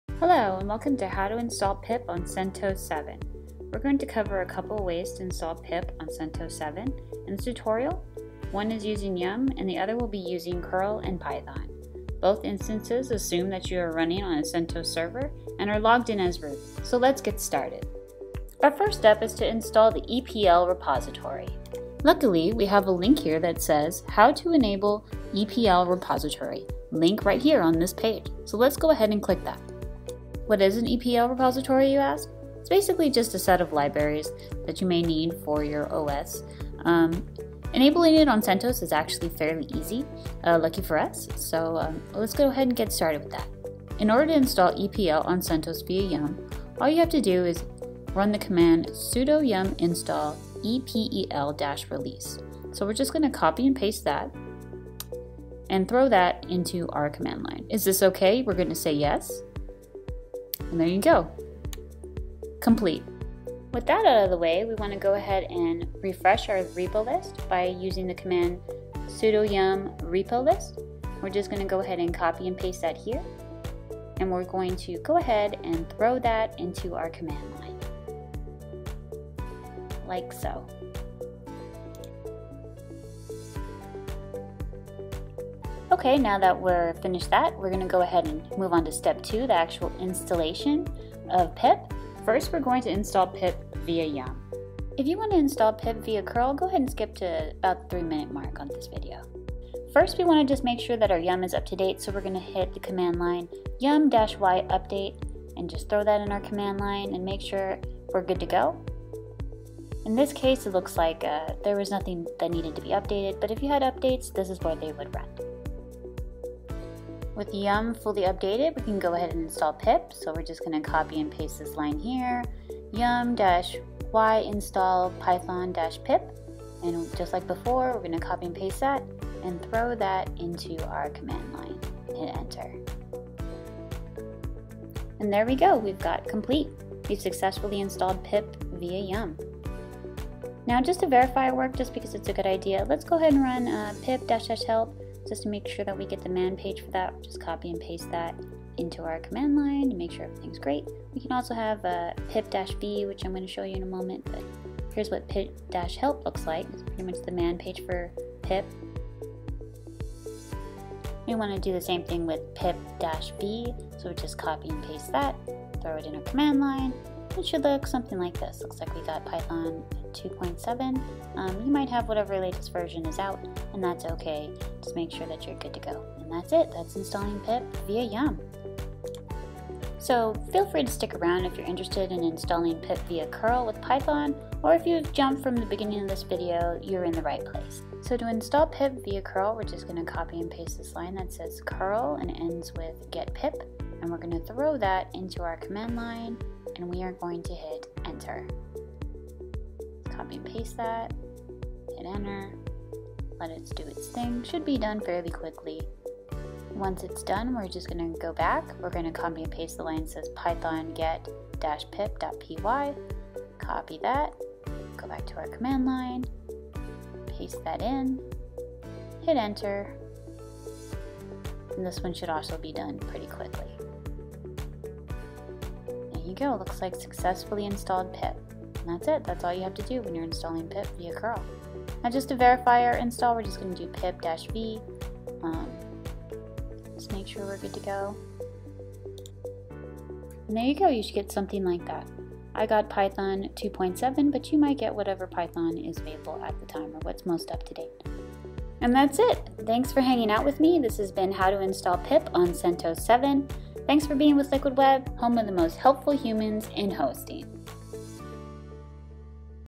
How to Install PIP on CentOS 7
Pip is a tool for installing and managing Python packages and is part of the Extra Packages for Enterprise Linux (EPEL), which is a community repository of non-standard packages for the RHEL distribution. In this video, we will review two ways to install it on CentOS 7.